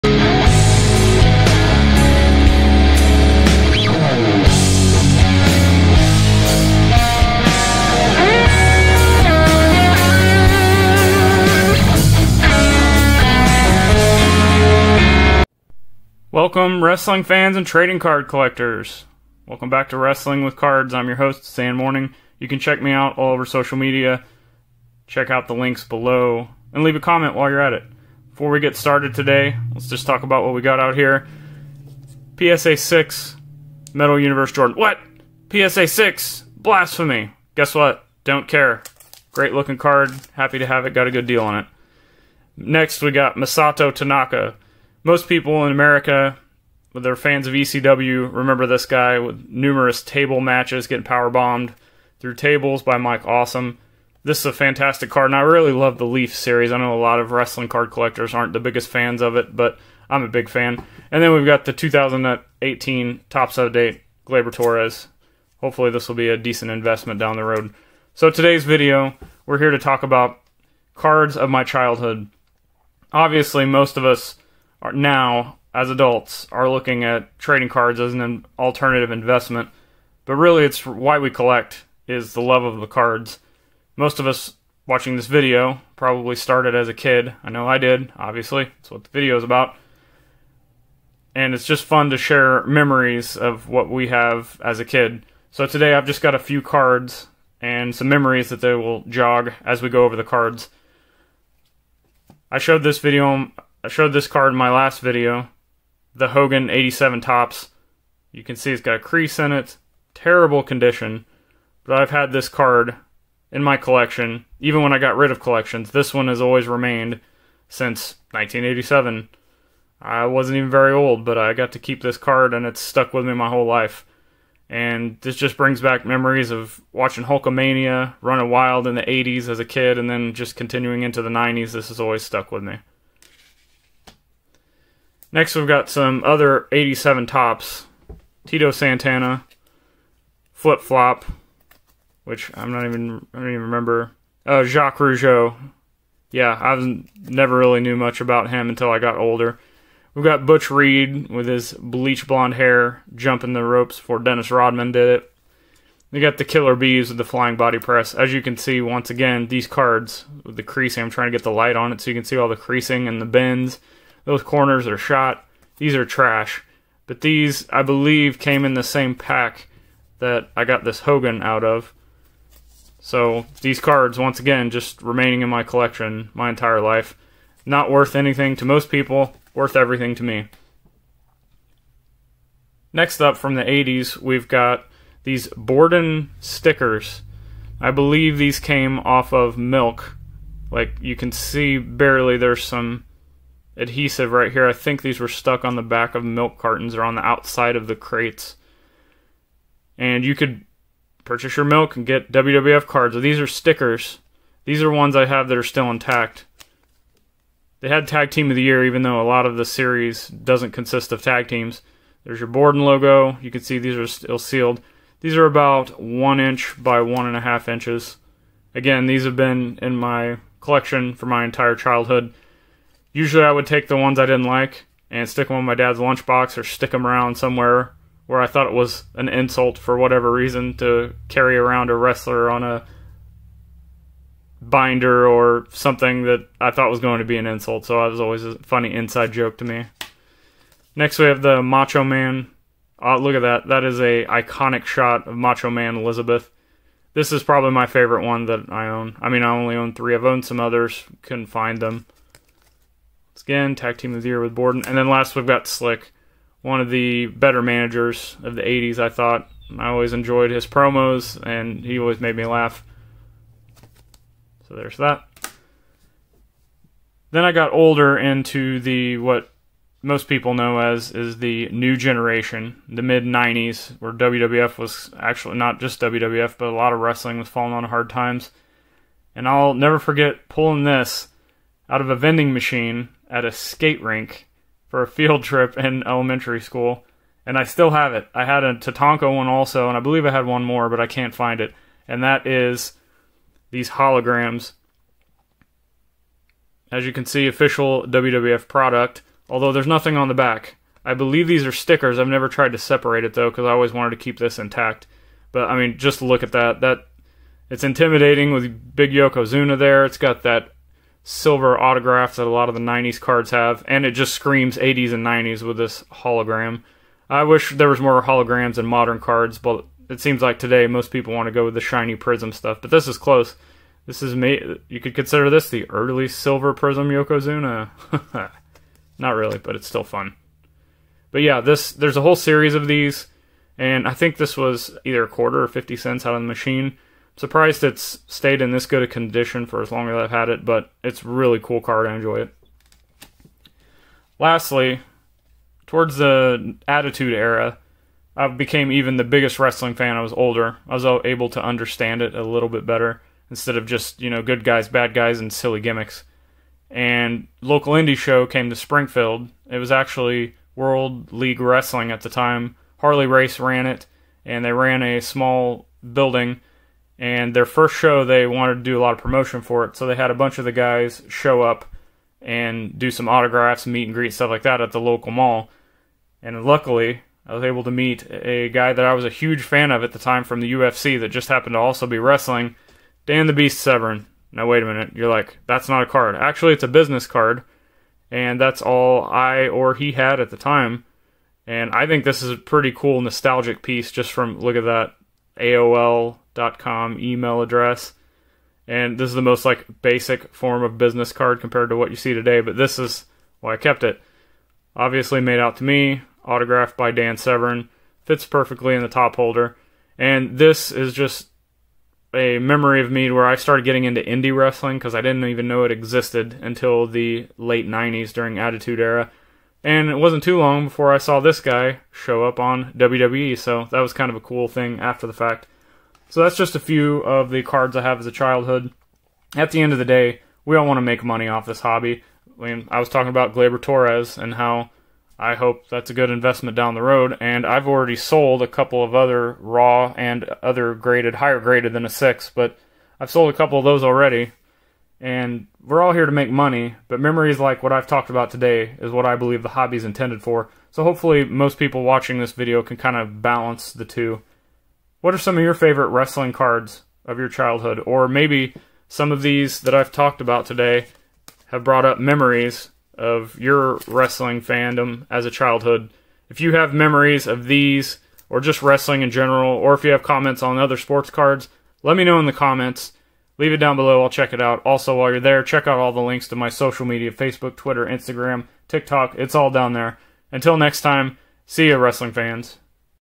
Wrestling Cards of My Childhood
Welcome to Wrestling with Cards!

In this video I discuss collecting Wrestling as a kid and some of the memories I still hold onto as an adult in the hobby of collecting Wrestling cards.

***Interested in showing your support?***